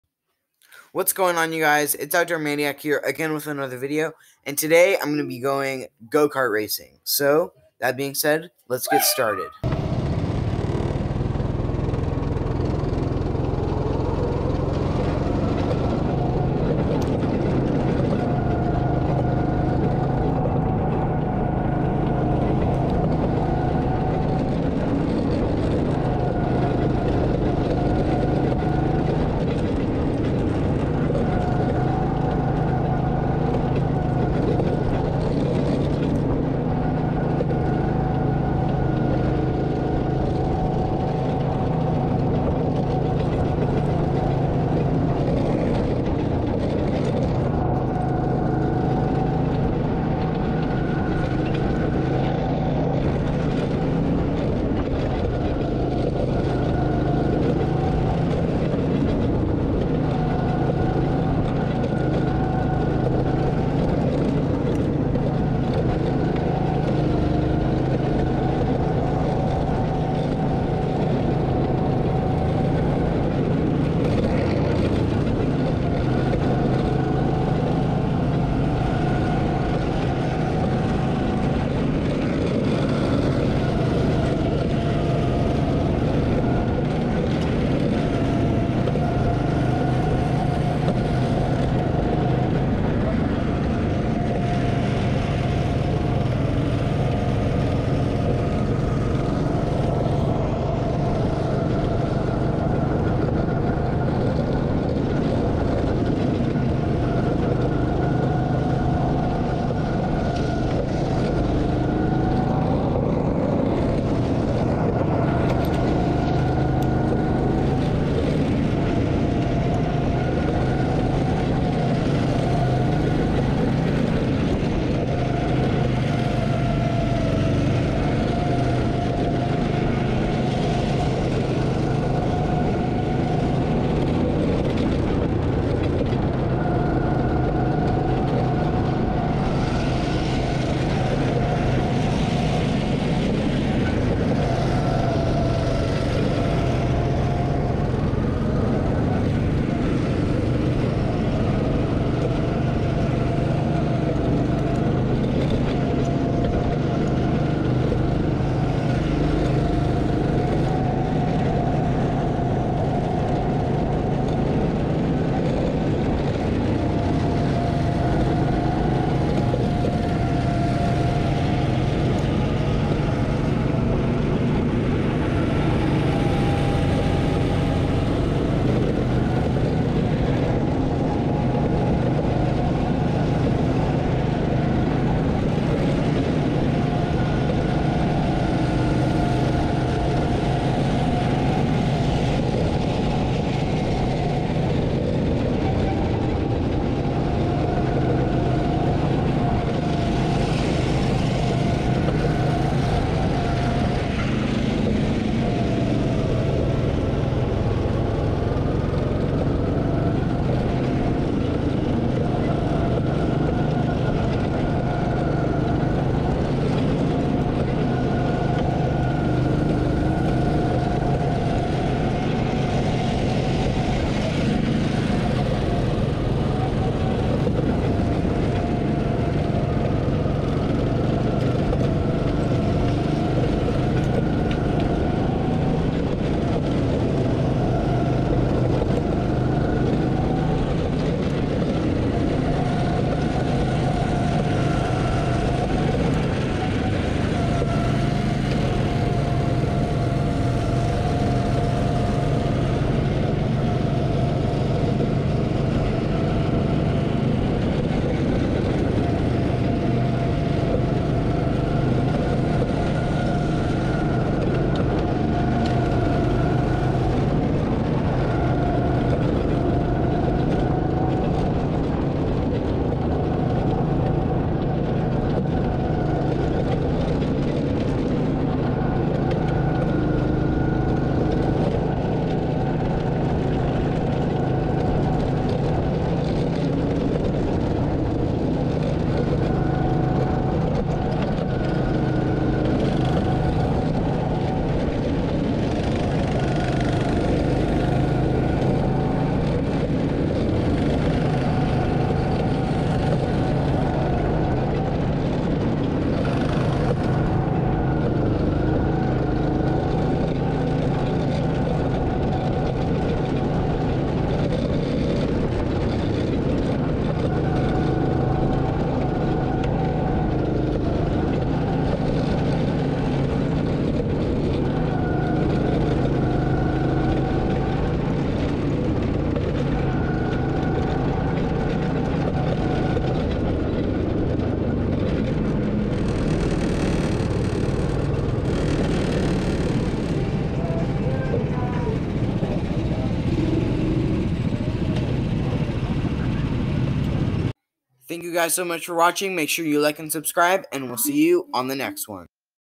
Go Kart Racing
In this video I go to a Family Fun Center in Yakima, WA to have some fun. Their will hopefully be a mini golf video that should should come out on